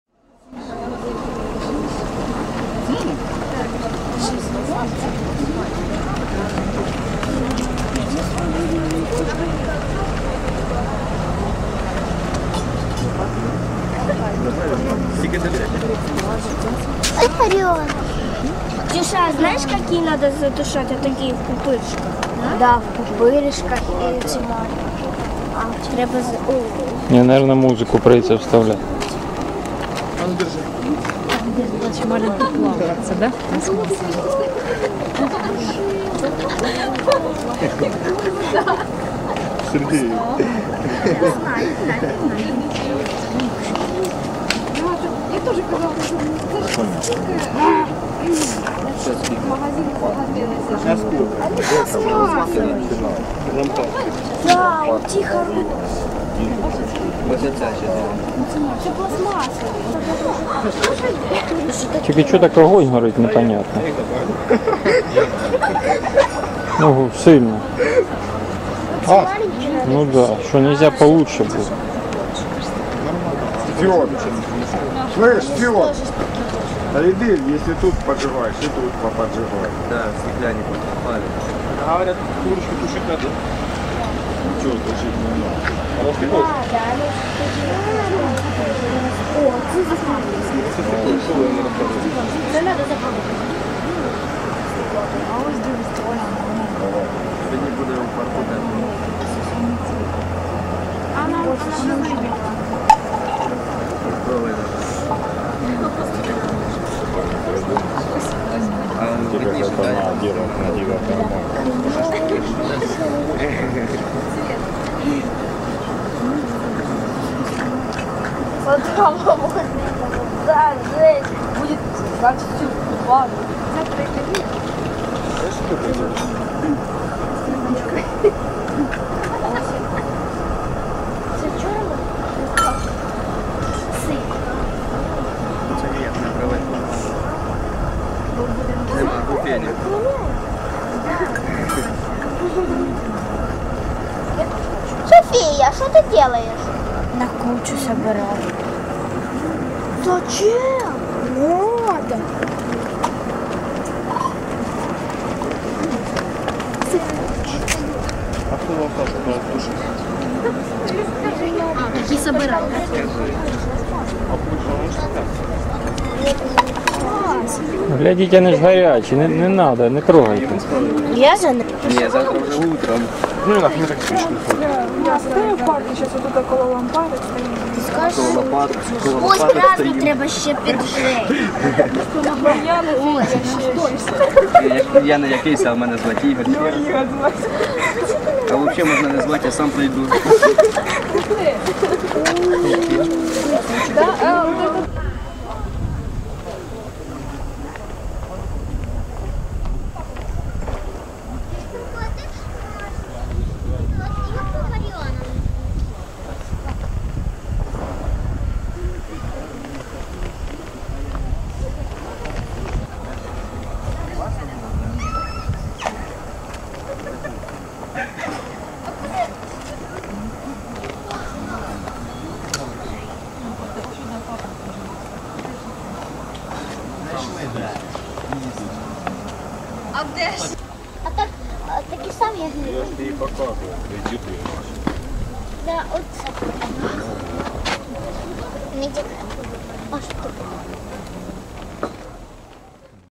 Свічки пам'яті в Білокуракине. Монумент Скорботна Мати, 06.05.2010
УКРАЇНА, Луганська область, Білокуракине, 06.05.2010 року.
Відео спеціально для білокуракинського порталу!